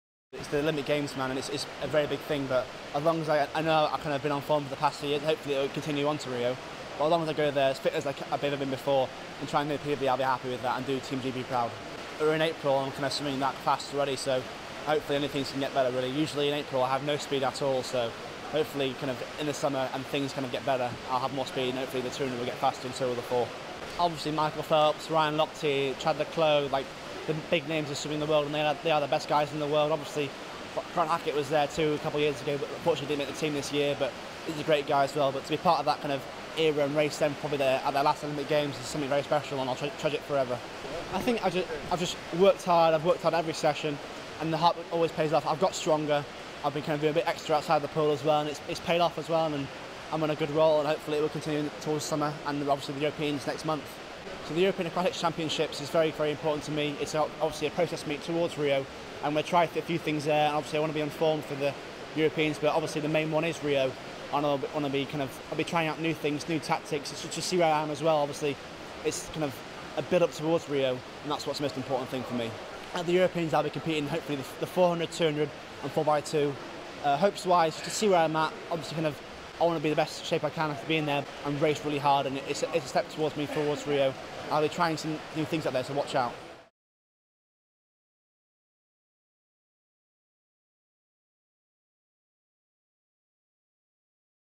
James Guy ready for European Aquatics Championships acid test
World Champion James Guy was relieved to book his place on the GB team heading to Rio this summer but insists the European Aquatics Championships is his next big test as he bids to compare himself to his rivals.